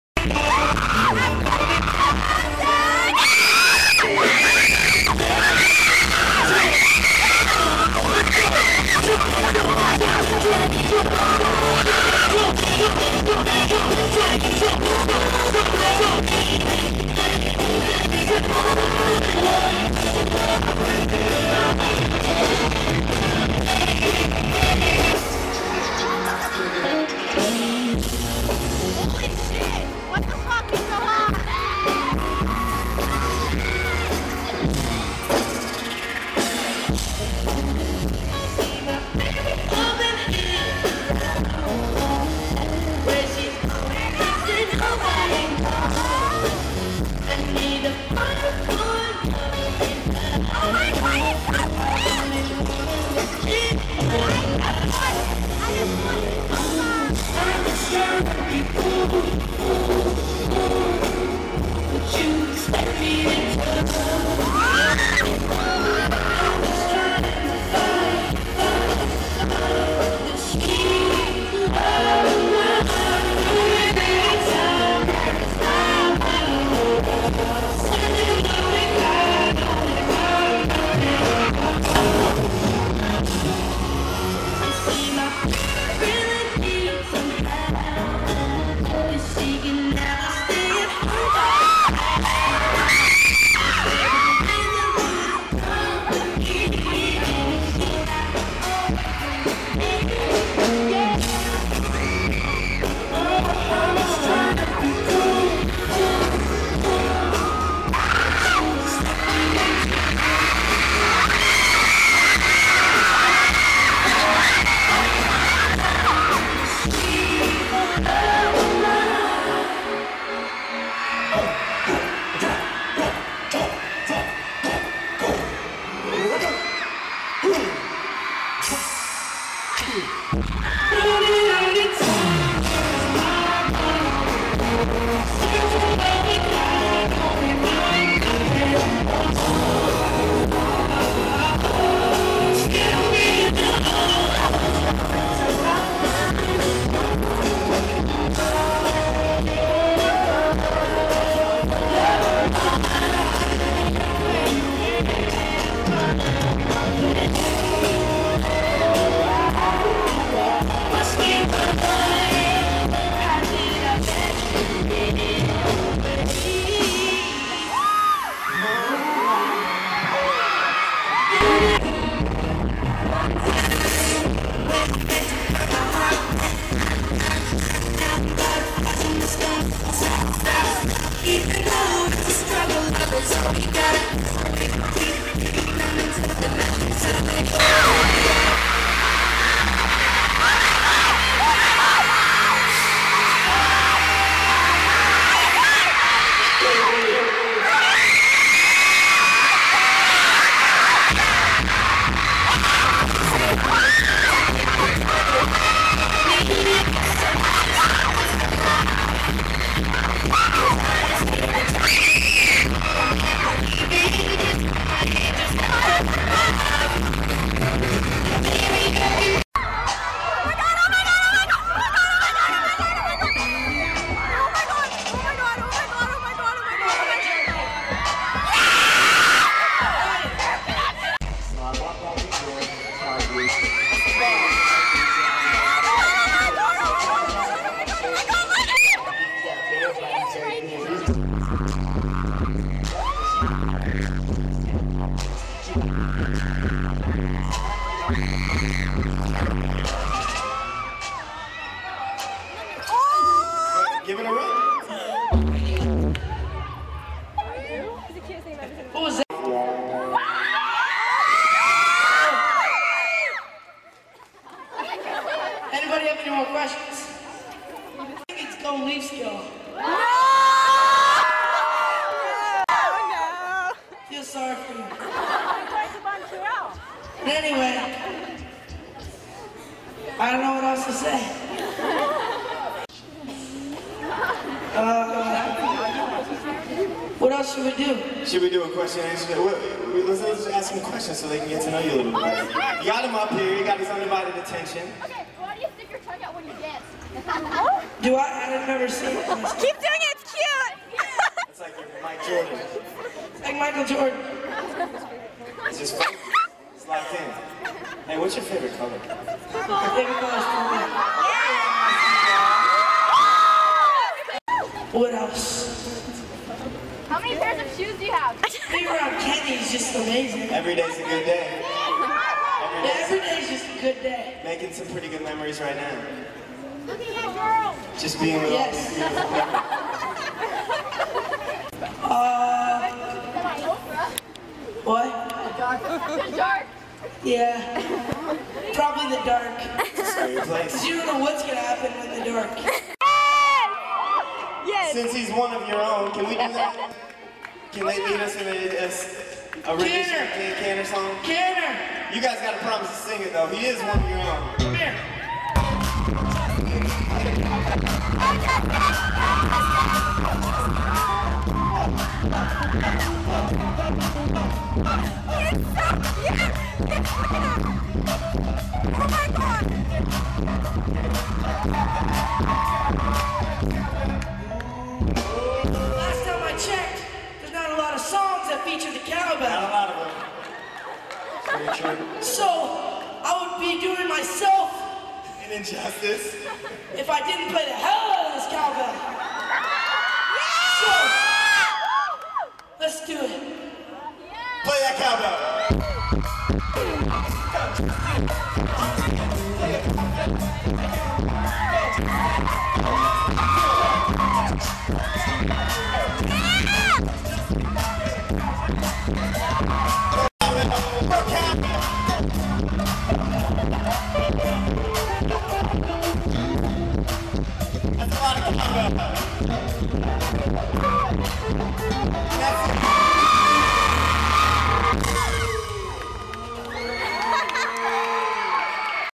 JUSTIN BIEBER MY WORLD TOUR 2010- OTTAWA CONCERT ! (AND SOUNDCHECK) !
I was fortunate enough to see Justin Bieber live for the first time last night (August 24th) at the Scotiabank Place in Ottawa. It was absolutely amazing & breath-taking!! I had SOOOOOOOO much fun, & I'd relive that night over & over & over again, if I had the choice. Here are just a few clips put together (I took like a kajillion videos) ahaha so I might make a part 2 ?! ...we'll see ! HOPE ALL YOU BELIEBERS ENJOY THIS AS MUCH AS I DID :)